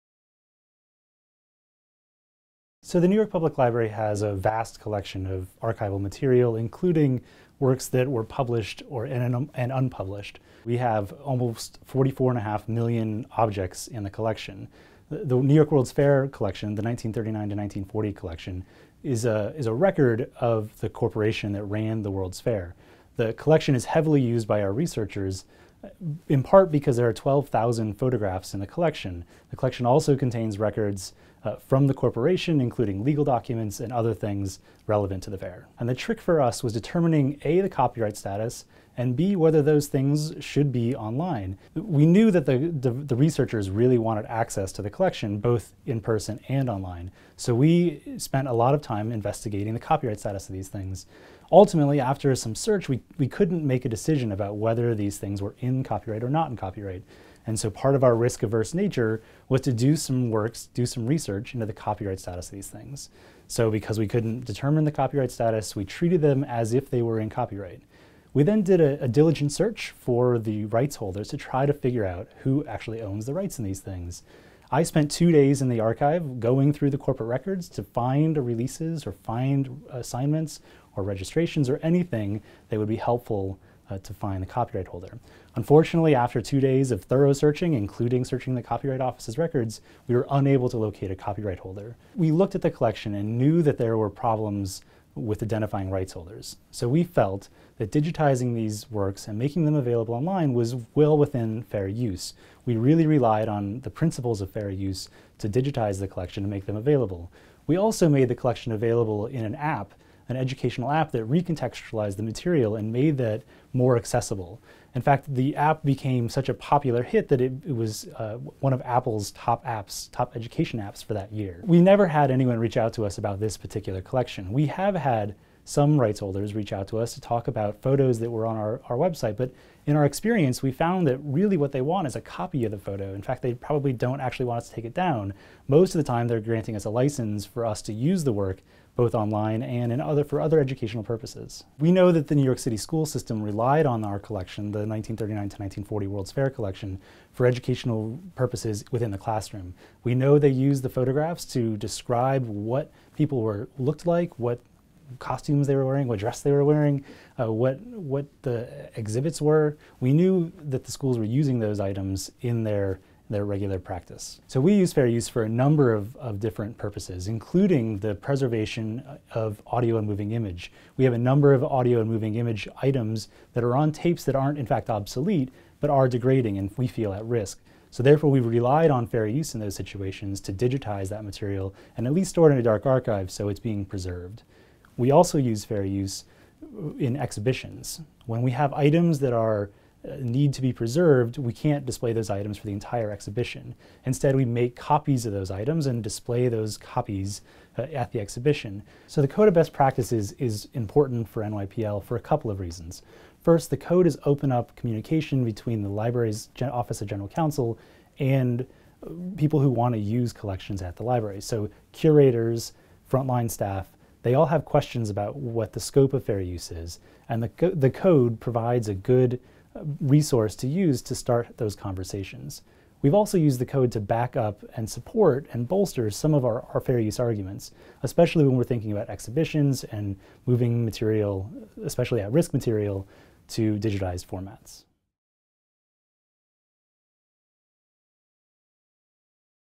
Code of Best Practices in Fair Use at the New York Public Library
In this four-minute video, Greg Cram of the New York Public Library discusses NYPL's application of the principles of fair use.

Greg Cram, associate director of copyright and information policy at the New York Public Library (NYPL), explains how NYPL utilizes the Code in support services and how, for example, the library used the Code in determining whether to digitize a large collection of materials from the 1939–1940 New York World’s Fair. An educational curriculum has been built around this collection and it is widely used in New York City schools. The materials have also been used in a free app, Biblion, that Apple named one of the best education apps of 2011.